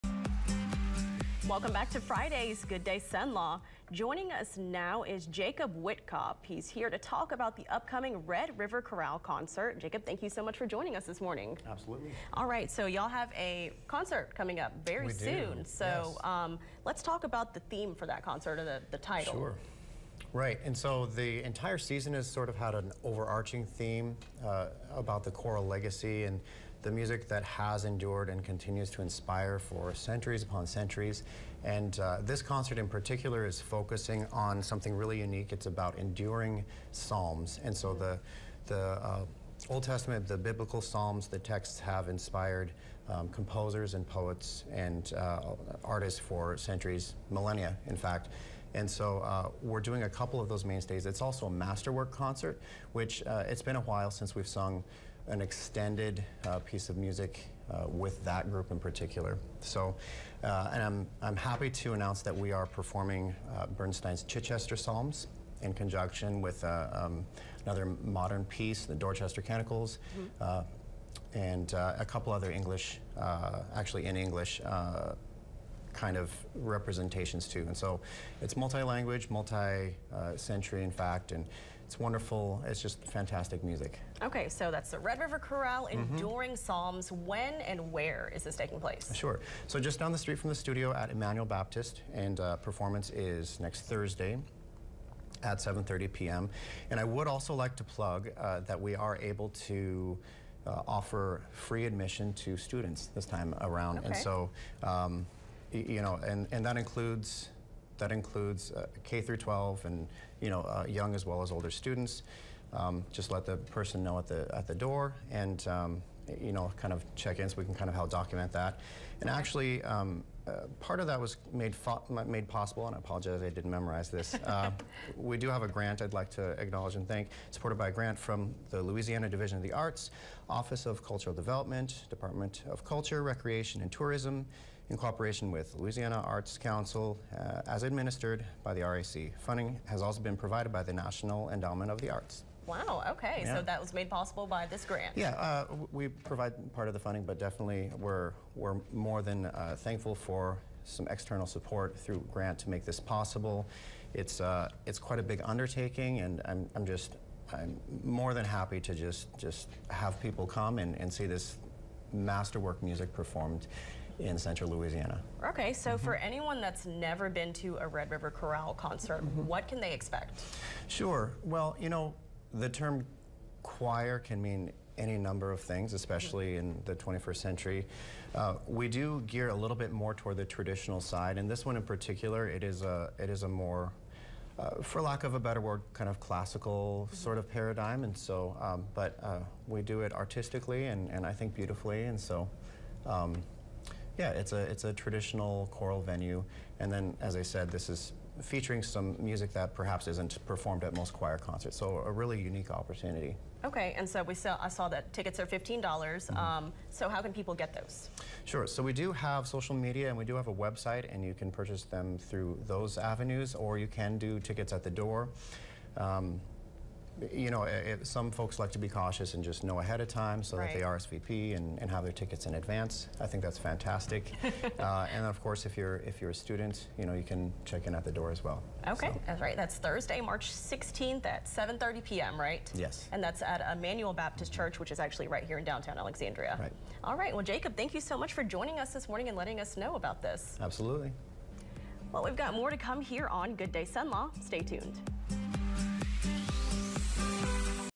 RRC Presents: 'Enduring Psalms'
Jacob Wittkopp previews an upcoming Red River Chorale concert